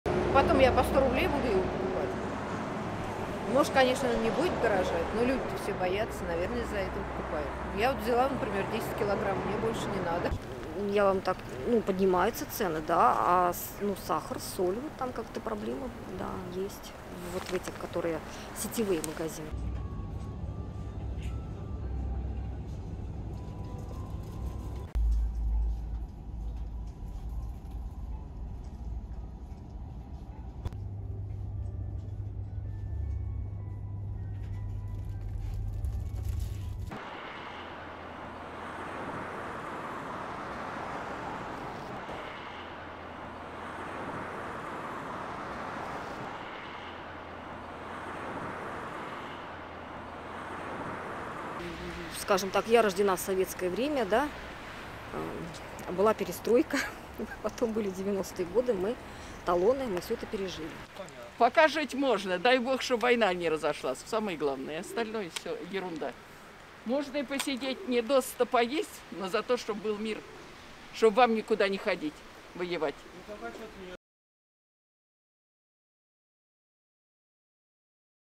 Sanctions impact Russia's food supply
“It’s fine not to eat one’s fill as long as there is peace.”

Western sanctions are starting to bite, and just outside Moscow, Russians are experiencing food shortages.